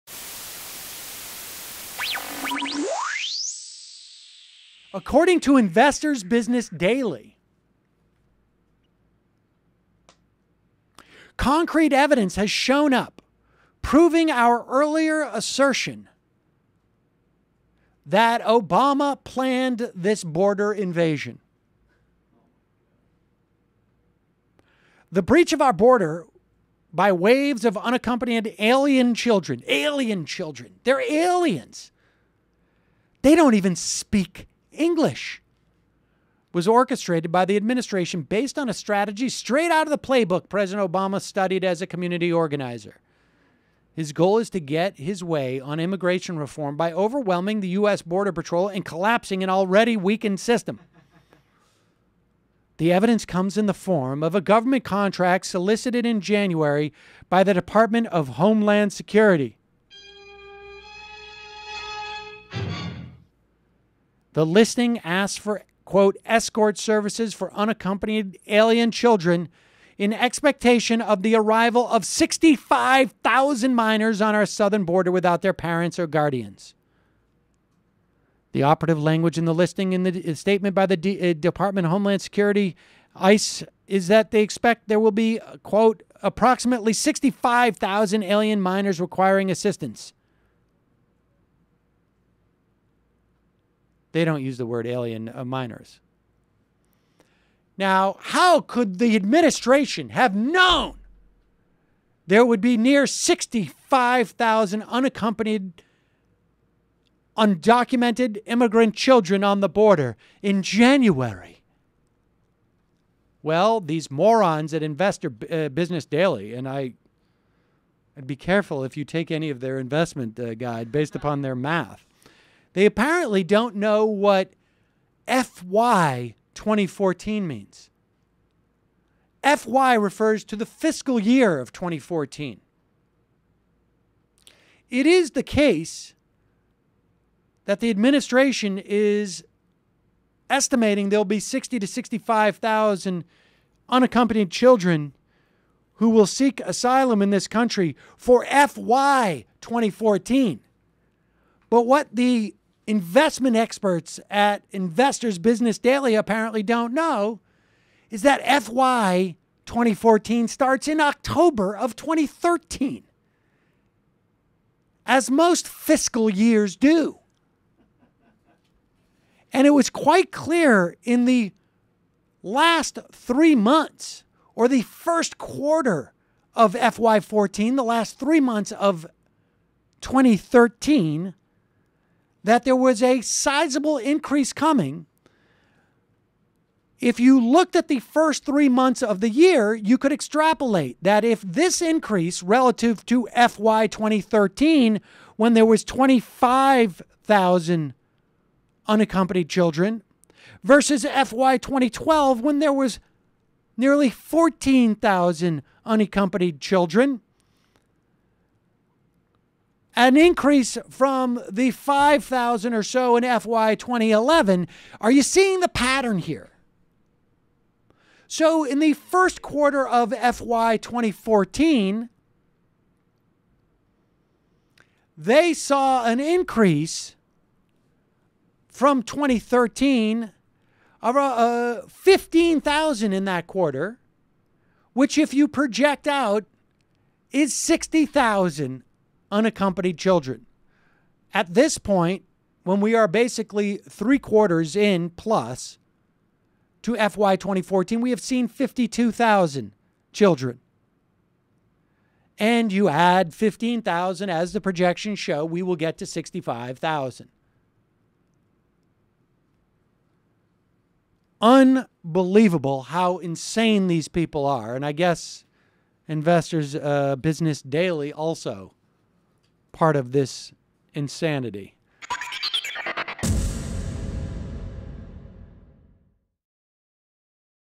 The Stupidest Immigration Conspiracy You'll Hear Today: Congrats Investor's Business Daily!
This might be the dumbest thing you hear about immigration today and it's all courtesy of Investor's Business Daily. No, President Obama is not "luring" immigrant children to cross our borders...